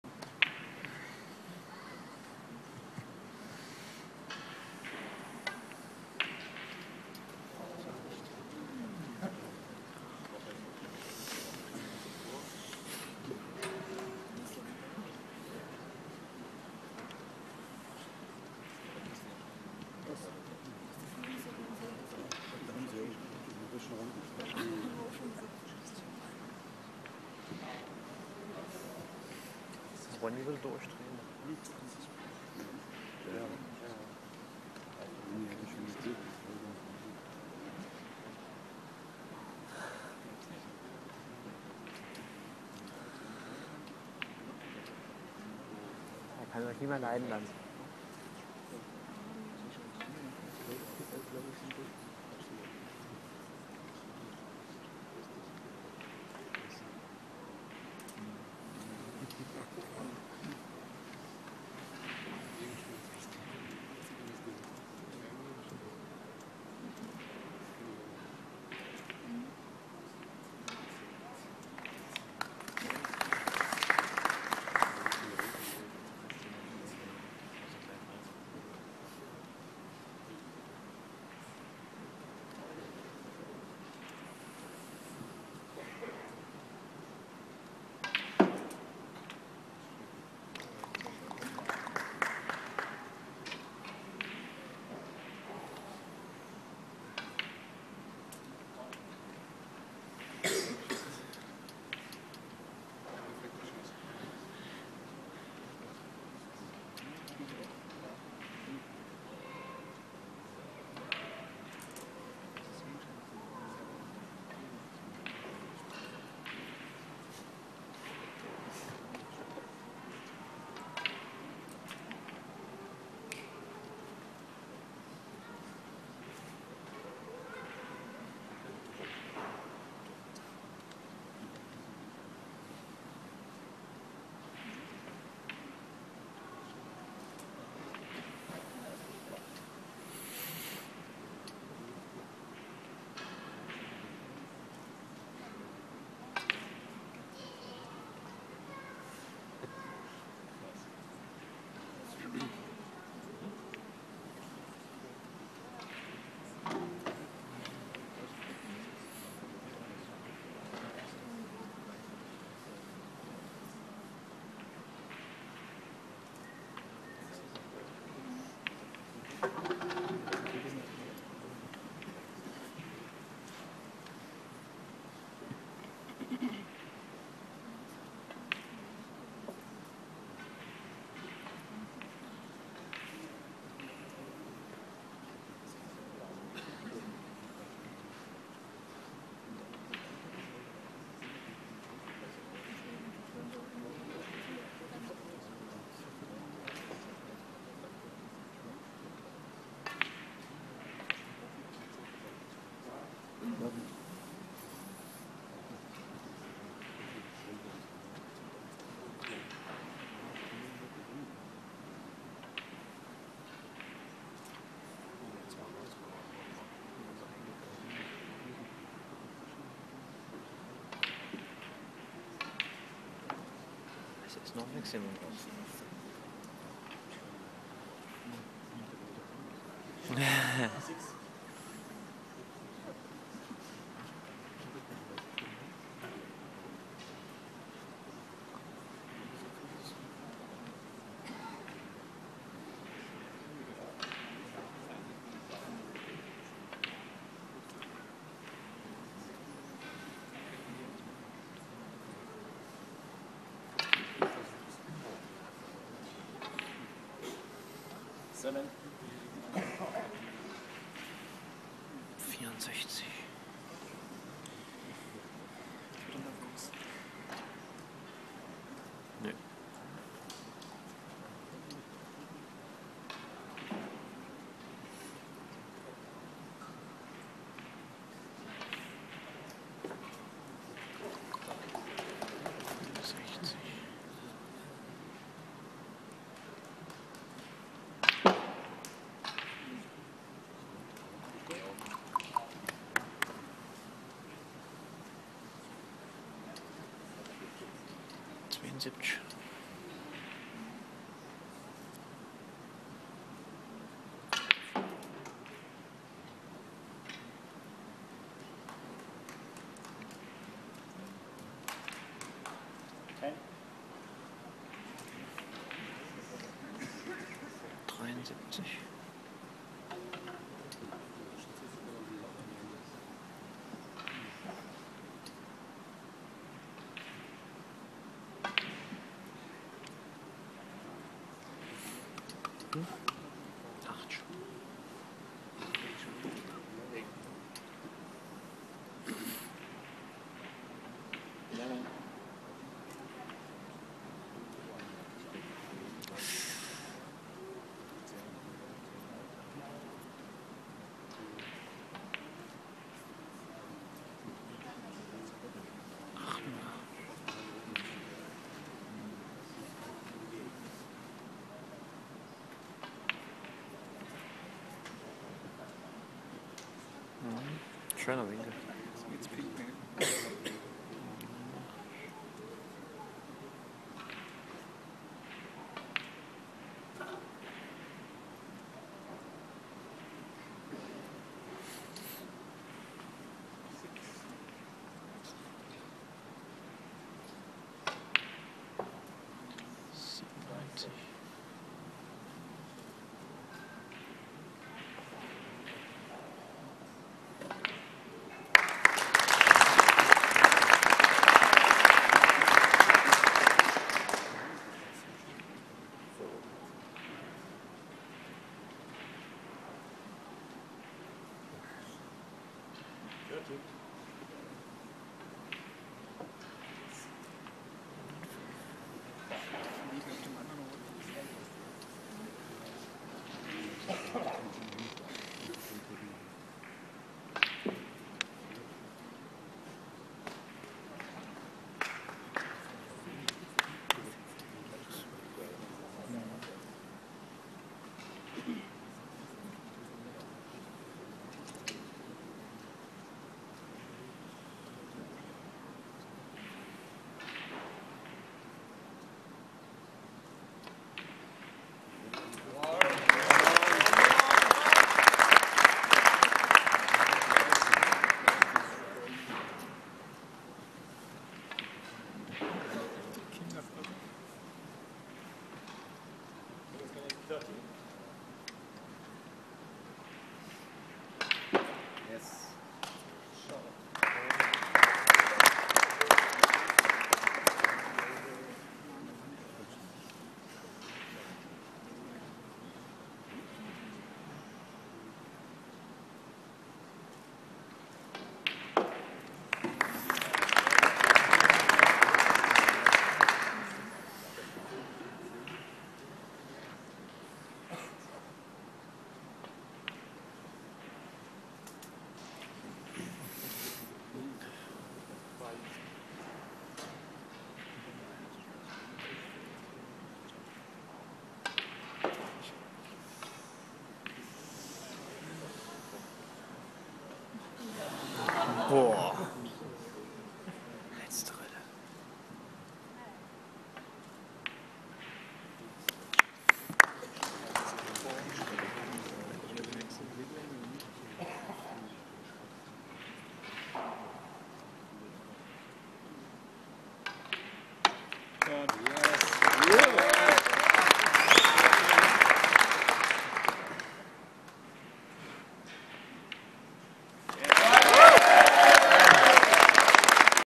Mark Williams - 147 Maximum Break - Austrian Snooker Open 2012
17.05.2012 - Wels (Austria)
Round Robin Stage